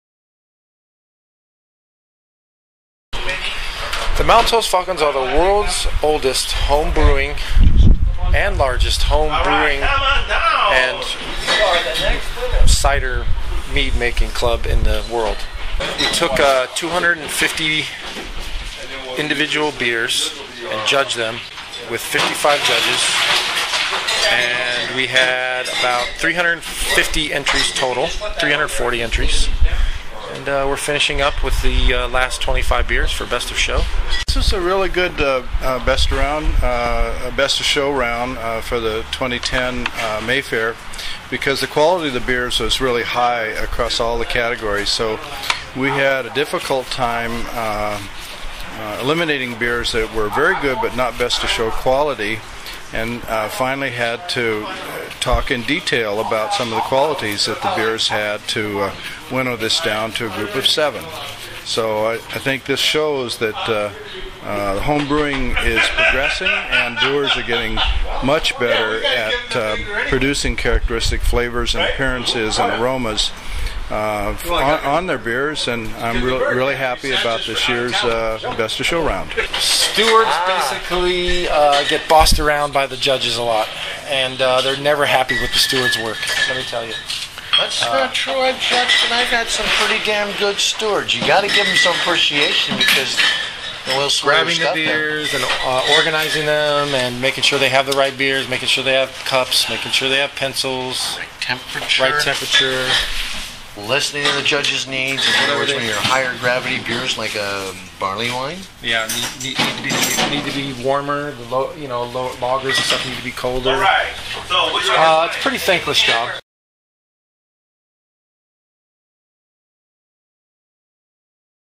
Maltose Falcons 2010 Mayfaire competition
The Maltose Falcons, America's oldest homebrewing club, held their 32nd annual Mayfaire homebrew competition at St. Martin in the Fields Episcopal Church in Winnetka, Calif., April 24, 2010.